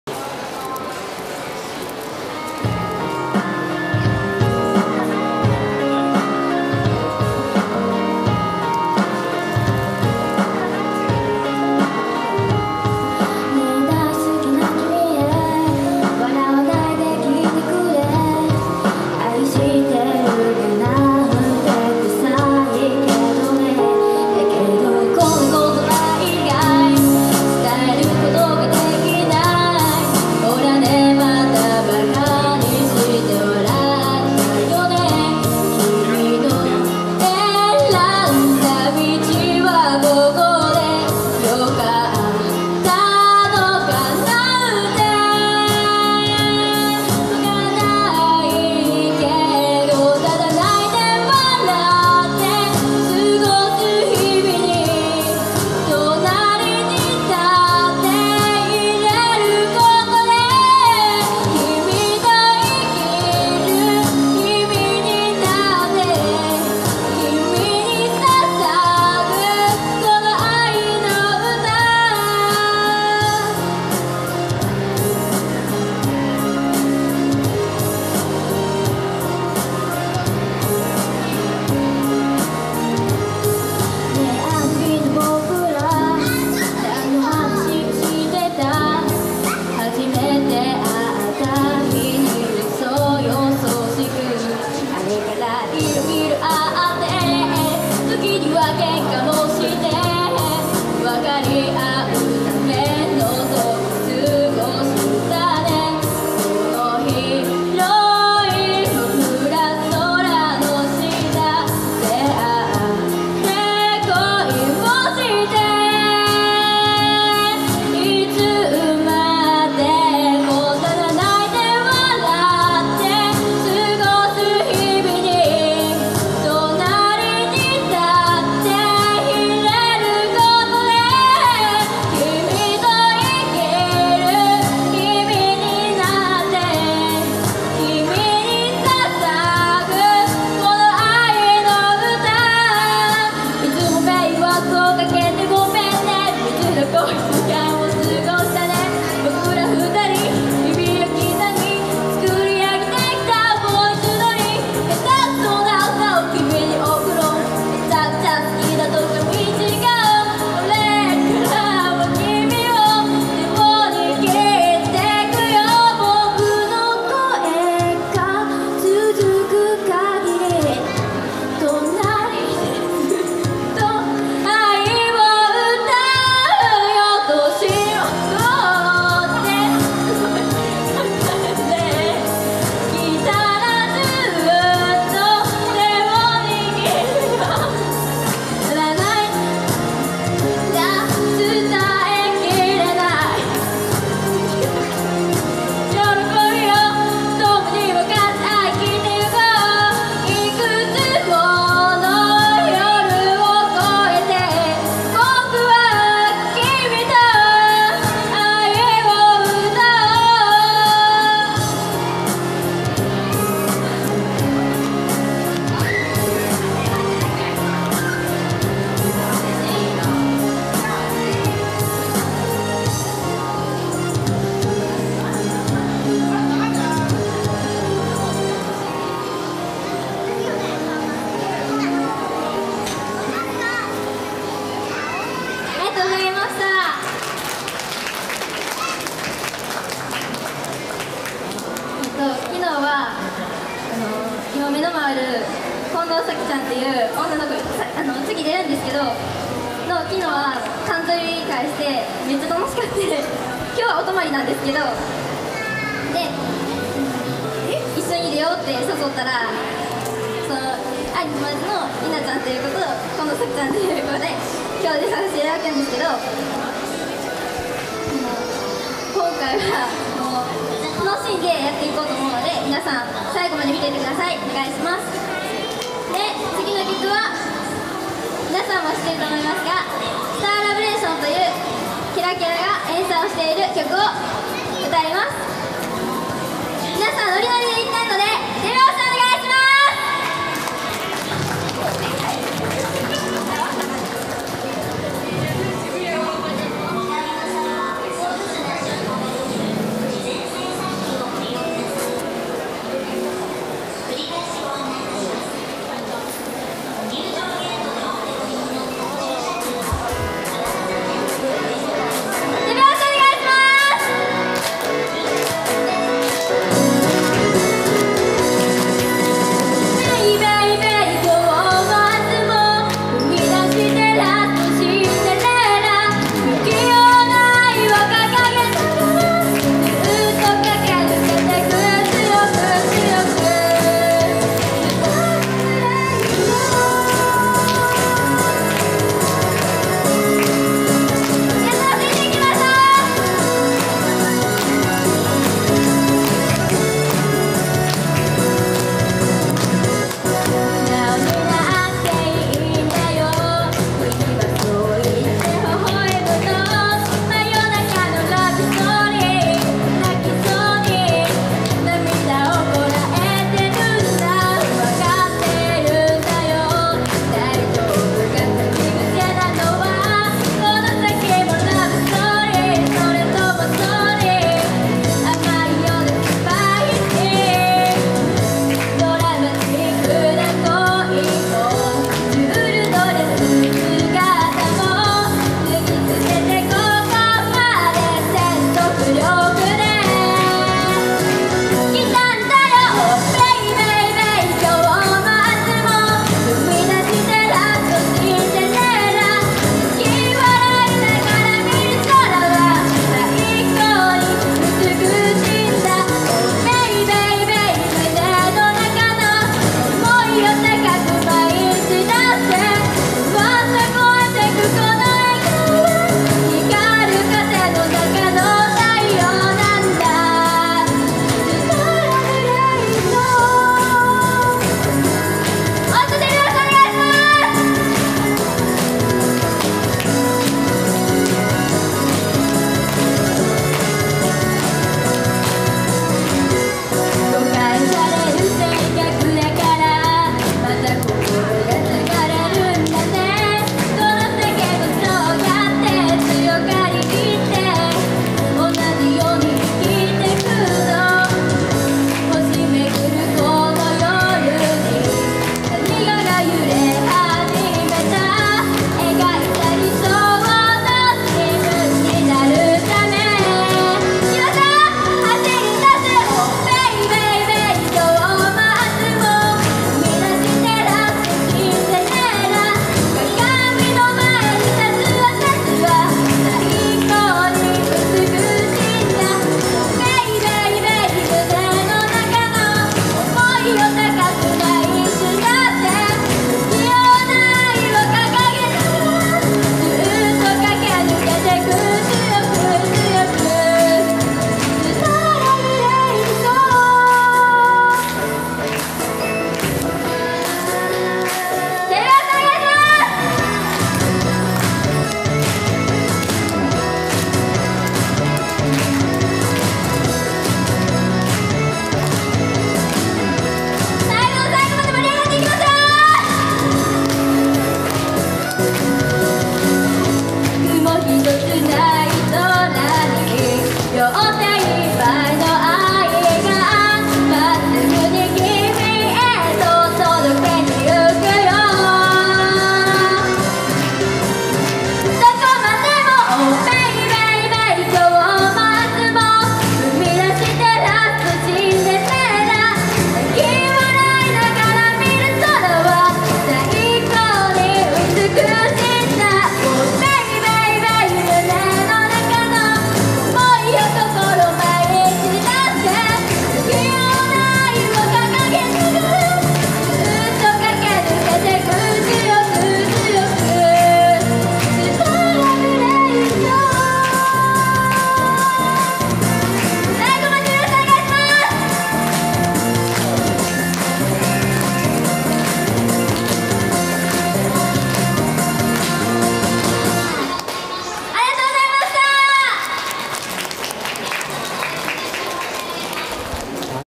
歌うたいパラダイス！くずは店 中村あいり
島村楽器くずはモール店歌うたいパラダイス！
中村あいりさんのパフォーマンスです！